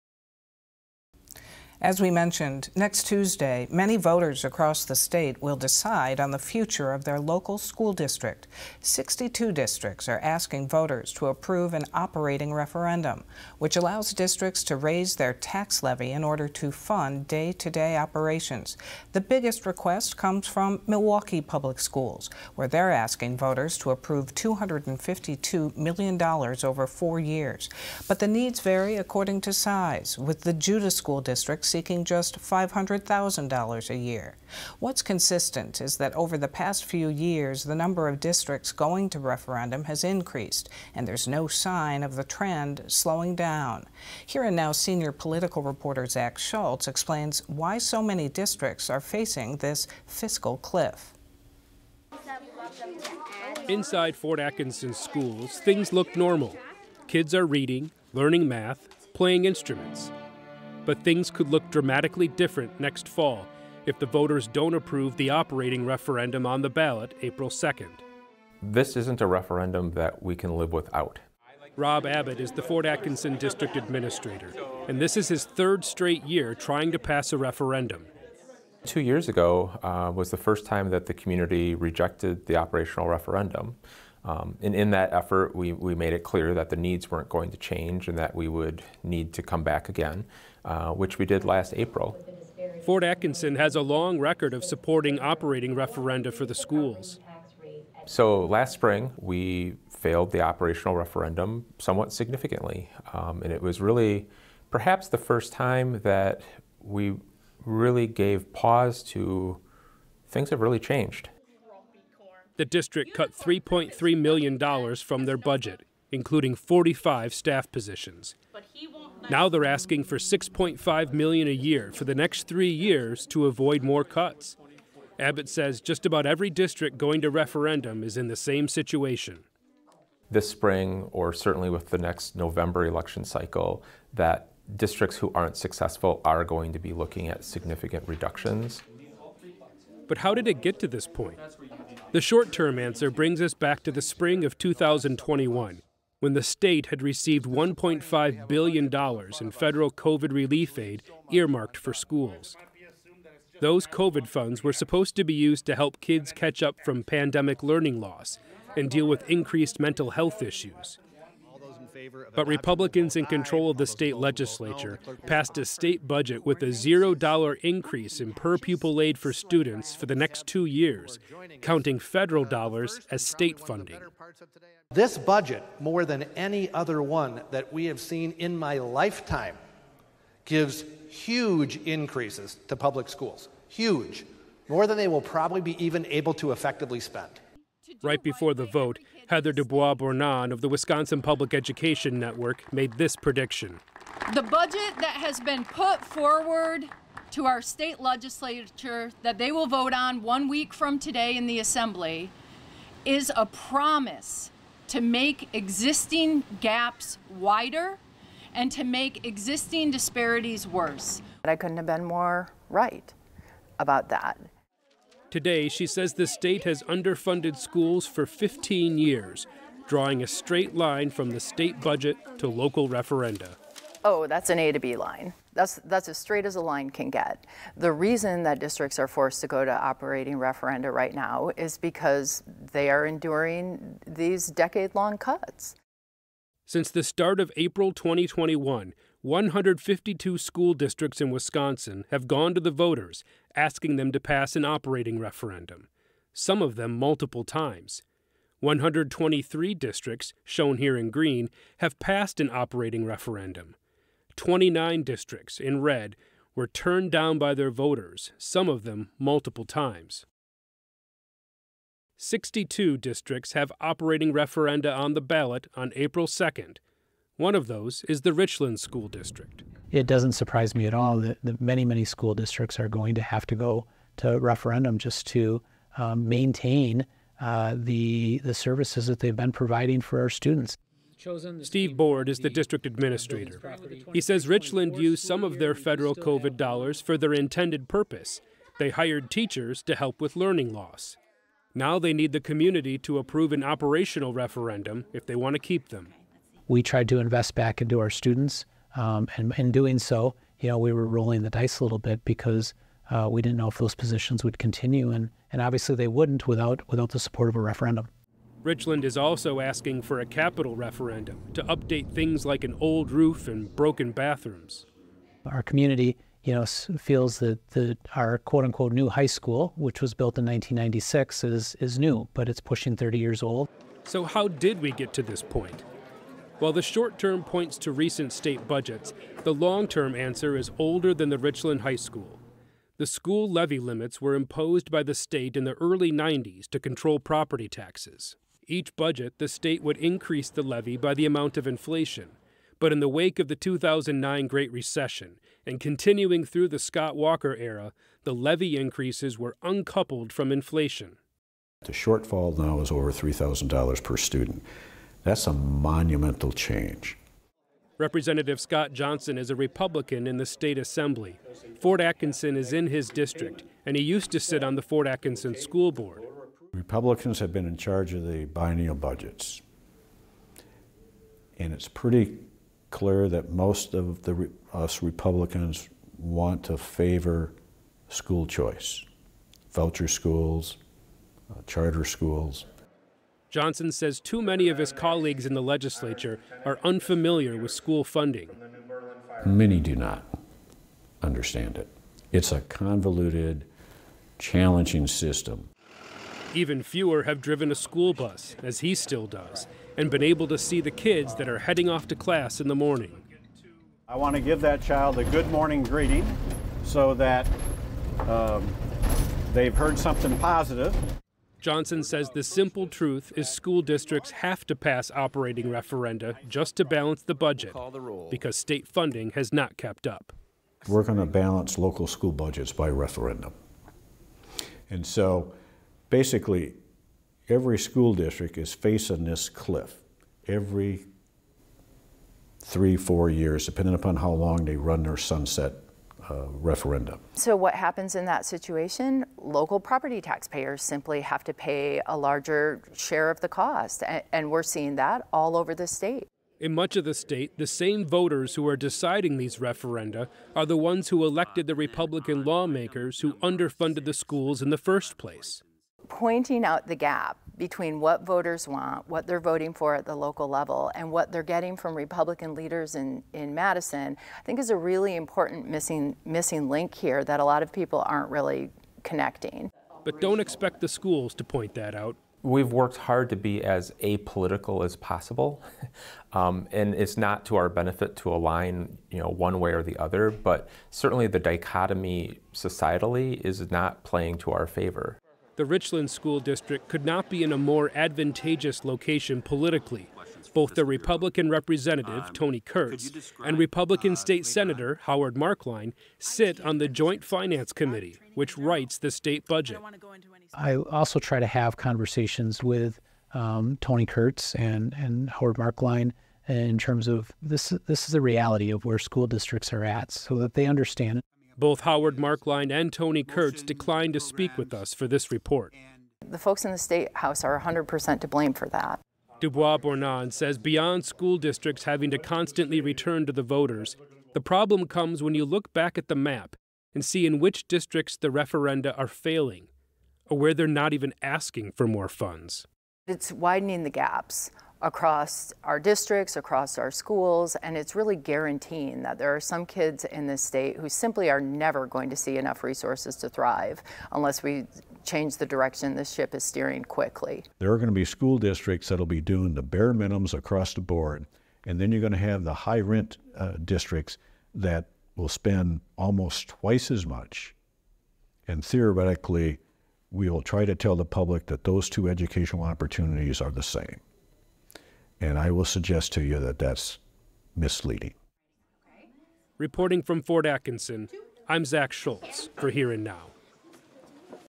Why are many Wisconsin school districts holding referendums?
Dozens of school districts around the state are going to referendum in the 2024 spring election, with local educators seeking support for spending priorities while working within state funding limits.